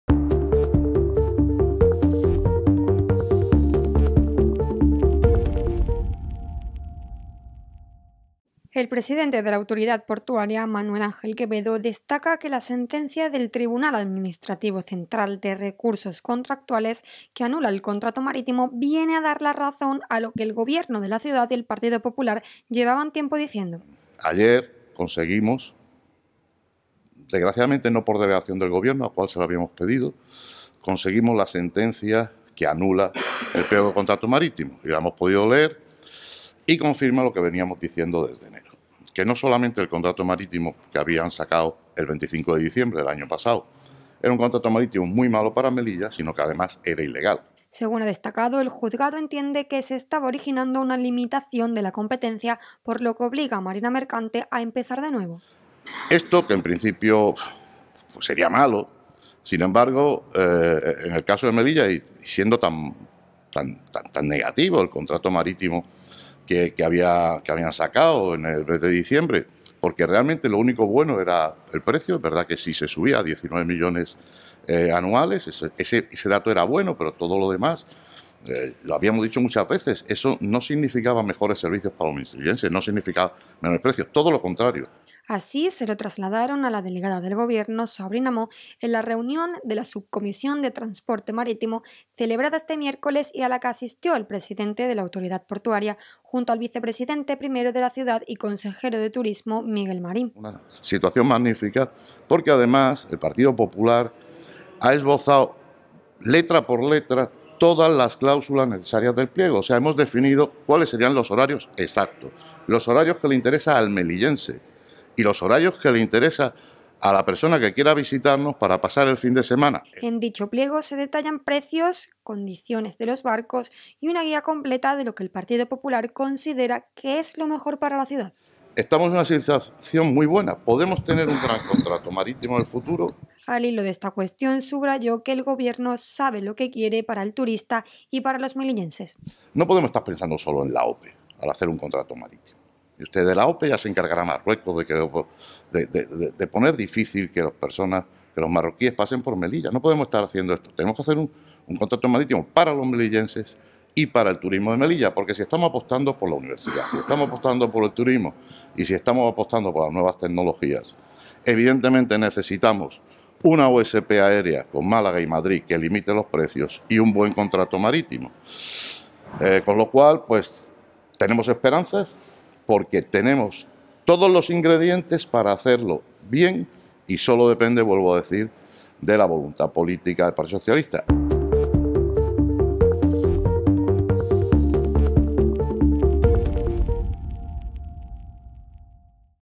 "La sentencia que anula el peor contrato marítimo de la historia confirma que era malo e ilegal"
El presidente de la Autoridad Portuaria, Manuel Ángel Quevedo, destaca que la sentencia del Tribunal Administrativo Central de Recursos Contractuales que anula el contrato marítimo viene a dar la razón a lo que el Gobierno de la Ciudad Autónoma y el Partido Popular llevaban tiempo diciendo (desde enero), que el acuerdo era "muy malo", pero que además era "ilegal", tal y como ha quedado patente.

Redacción y locución: Miriam González